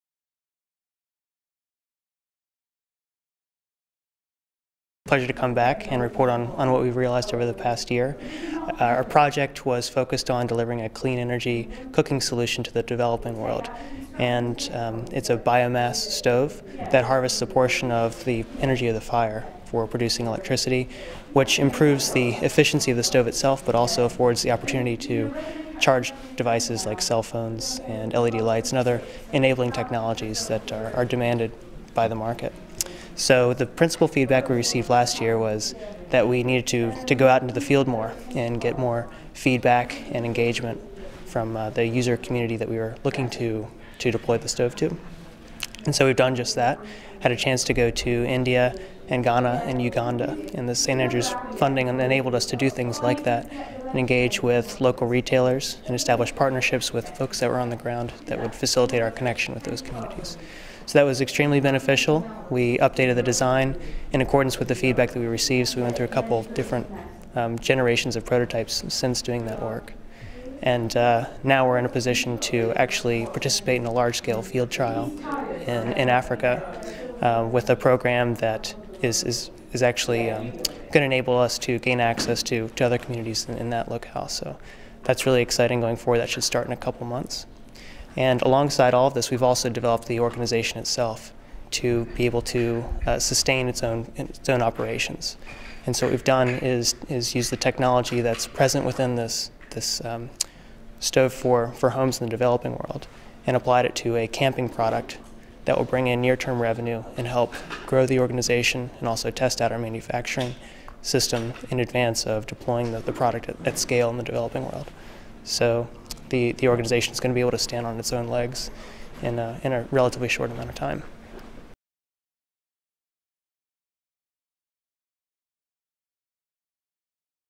Jonathan den Hartog of BioLite - 2012 St Andrews Prize for the Environment
The BioLite project won the St Andrews Prize for the Environment project in 2011. Jonathan den Hartog of BioLite provides an update on the development of the BioLite product since winning the prize in 2011. The interview was held at the 2012 awards at University of St Andrews, held on the 25th of April 2012,

To find out more about the St Andrews Prize for the Environment